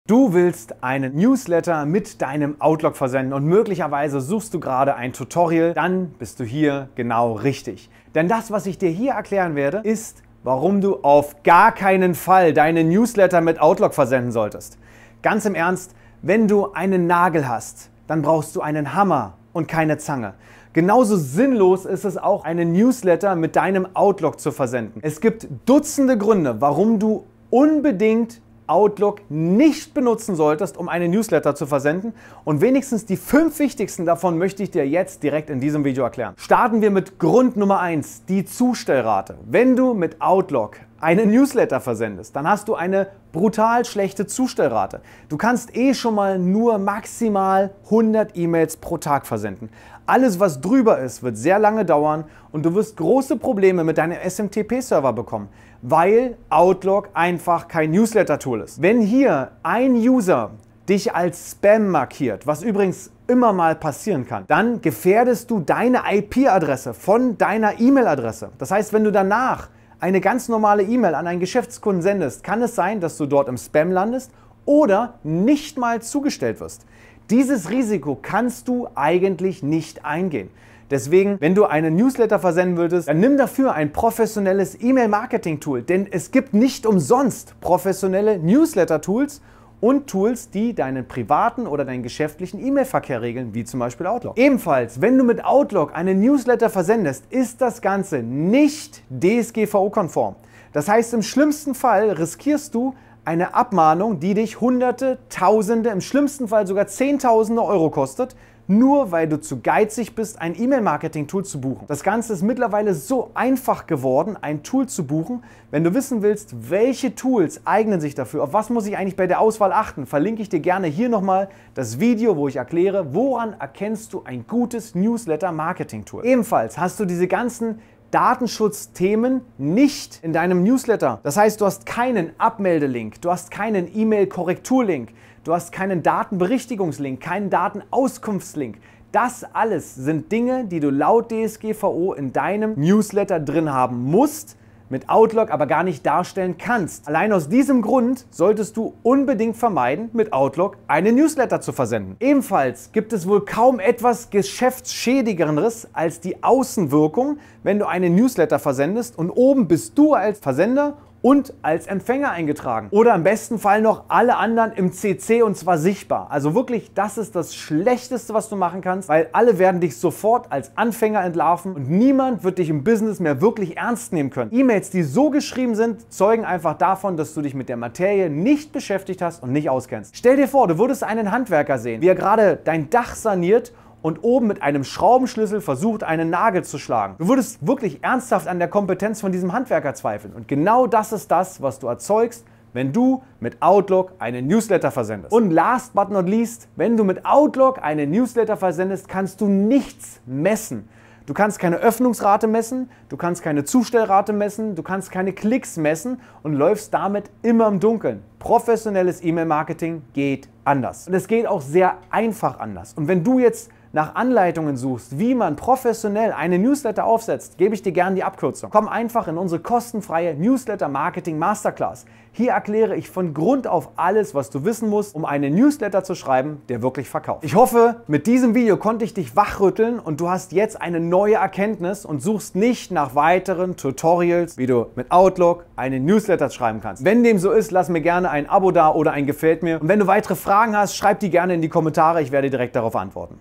Newsletter mit Outlook verschicken - Einsteiger-Tutorial
Du willst einen Newsletter mit deinem Office 365 Outlook versenden?
Perfekt - dann solltest du dieses Video unbedingt sehen.

Es ist die einzige Erklärung, die du brauchst.
Denn es erklärt, warum du wirklich absolut niemals Outlook verwenden solltest, um deinen Newsletter zu versenden.

Es gibt zahlreiche Gründe, die dagegen sprechen. Zum einen ist die Zustellrate bei Outlook sehr schlecht, da du nur eine begrenzte Anzahl von E-Mails pro Tag versenden kannst. Das kann zu großen Problemen mit deinem SMTP-Server führen und deine IP-Adresse gefährden.

Des Weiteren ist Outlook nicht DSGVO-konform und kann im schlimmsten Fall zu teuren Abmahnungen führen. Außerdem fehlen wichtige Datenschutzfunktionen wie Abmeldelinks oder Datenberichtigungslinks.

Ein weiterer Nachteil ist die mangelnde Professionalität. Wenn du Newsletter über Outlook verschickst und alle Empfänger im CC einträgst, wirkst du schnell unprofessionell und kannst von Geschäftspartnern nicht ernst genommen werden.

Zu guter Letzt kannst du mit Outlook keine Erfolgsstatistiken messen, wie Öffnungs- oder Klickraten. Das macht professionelles E-Mail-Marketing nahezu unmöglich.

Wenn du also wirklich erfolgreiche Newsletter versenden möchtest, solltest du auf professionelle E-Mail-Marketing-Tools setzen. In unserer kostenlosen Newsletter-Marketing-Masterclass erkläre ich dir alles, was du wissen musst, um erfolgreiche Newsletter zu schreiben.

Ich hoffe, dieses Video hat dich überzeugt, nicht weiter nach Tutorials für das Versenden von Newslettern mit Outlook zu suchen. Wenn du weitere Fragen hast, hinterlasse sie gerne in den Kommentaren - ich antworte dir persönlich. Bis bald!Newsletter mit Outlook

Outlook Newsletter Anleitung
Outlook Newsletter versenden Tutorial
Outlook newsletter tutorial